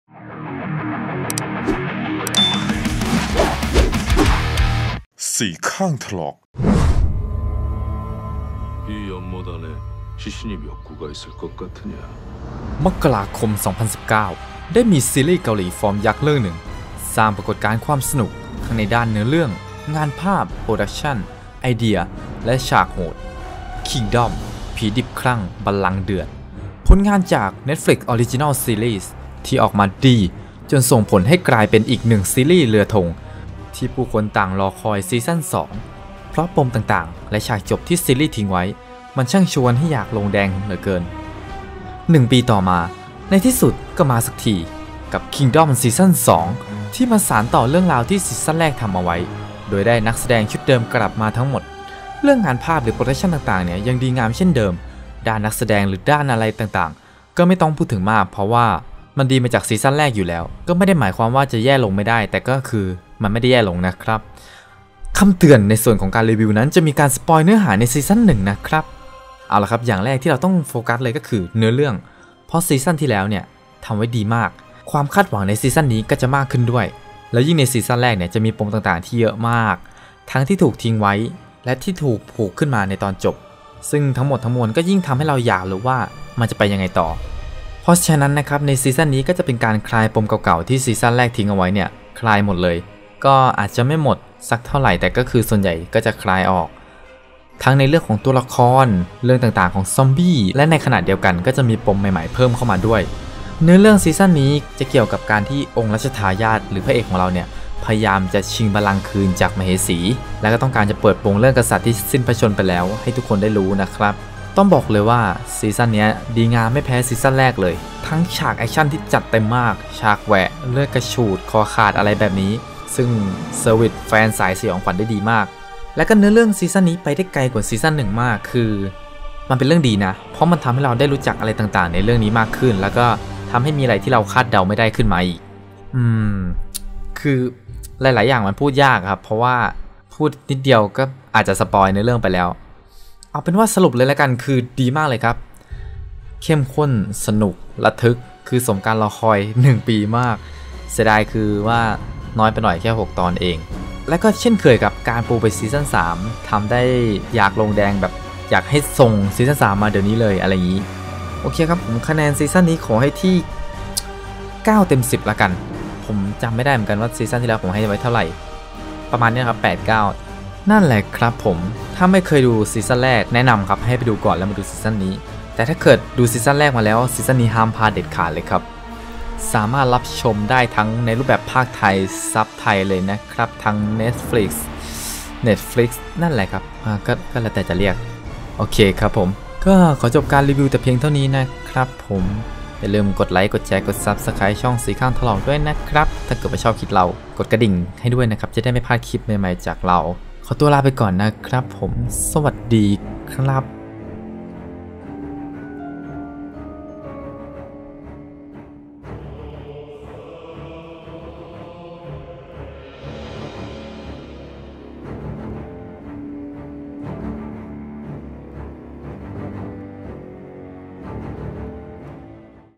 รีวิวซีรีส์ ผีดิบคลั่ง บัลลังก์เดือด ซีซั่น 2 (Kingdom Season 2) | NETFLIX
ซีรีส์ ผีดิบคลั่ง บัลลังก์เดือด (Kingdom) Season 2

📽 หากคุณชอบคลิปเราสามารถกดไลค์ กดติดตาม และกดแจ้งเตือนเพื่อติดตามคลิปใหม่ๆได้ที่ สีข้างถลอก 🎥